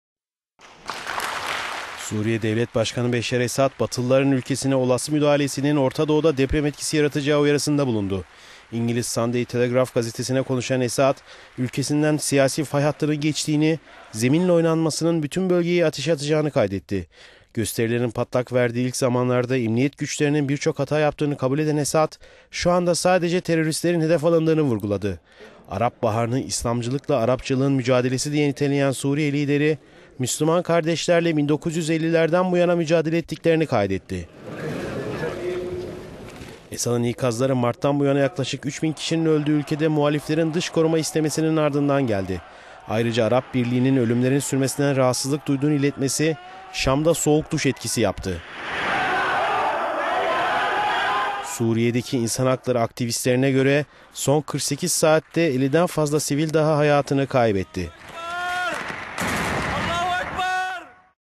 Esad'dan Batı'ya uyarı
Suriye Devlet Başkanı Beşşar Esad, Batılılar'ın ülkesine olası müdahalesinin Ortadoğu'da deprem etkisi yaratacağı uyarısında bulundu. İngiliz Sunday Telegraph gazetesine konuşan Esad, ülkesinden siyasi fay hattının geçtiğini, zeminle oynanmasının bütün bölgeyi ateşe atacağını kaydetti. Gösterilerin patlak verdiği ilk zamanlarda emniyet güçlerinin birçok hata yaptığını kabul eden Esad, şu anda sadece teröristlerin hedef alındığını vurguladı. Arap Baharı'nı İslamcılık ile Arapçılığın mücadelesi diye niteleyen Sur